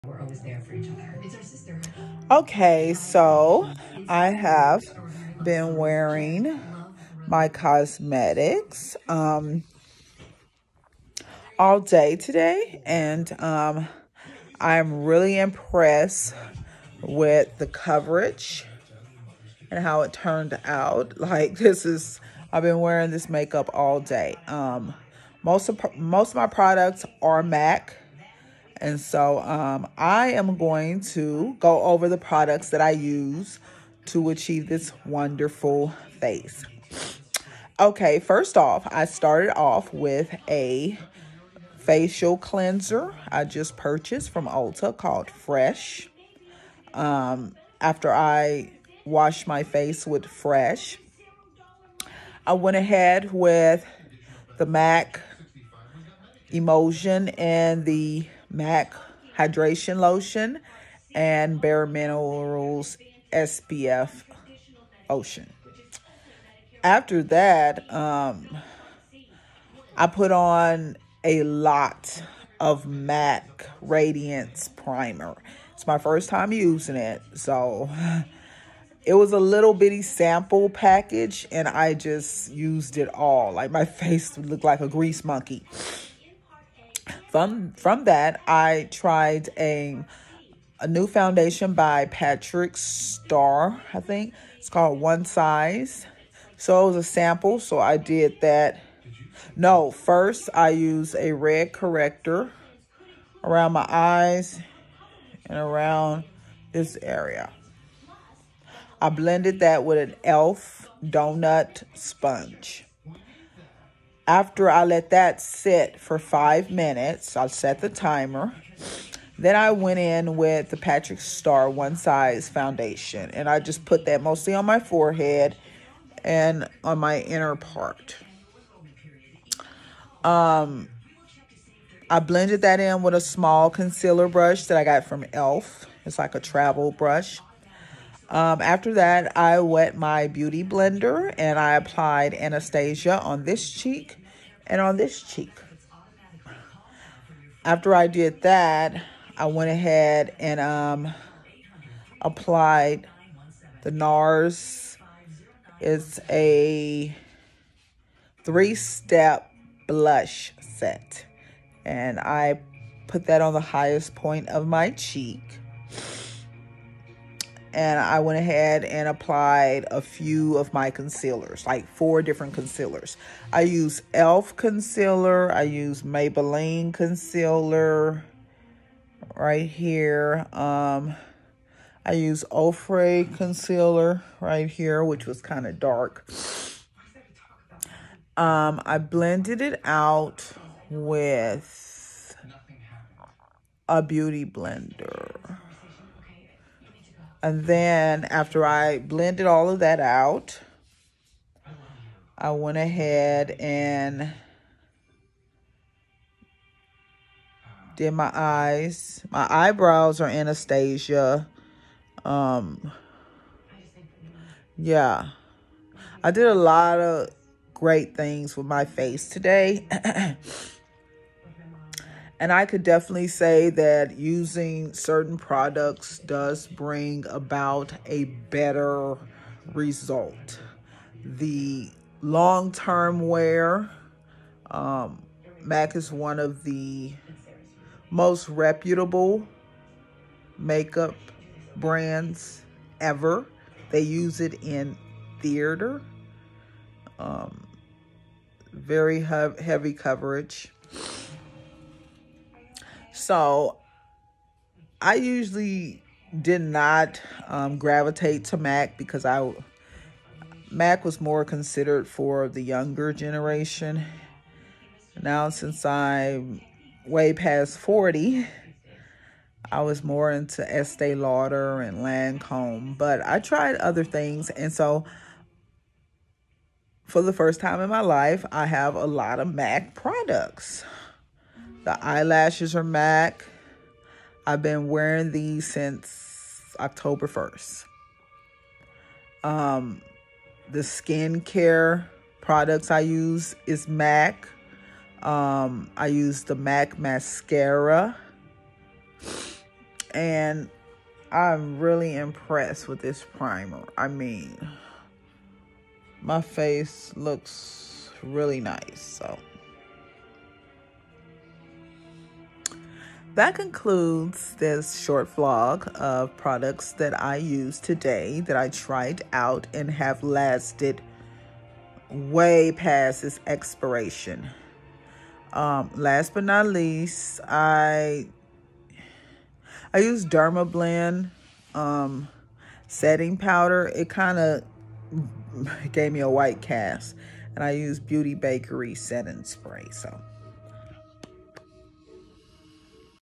MAC skincare line is BOMB
Short chit chat vlog of skincare products great. Including MAC hydration lotion, and emulsion. Great combo on caramelskin matureskinmakeup AnastasiaFoundation buildable medium coverage. Please share your skincare and cosmetic experience for over 40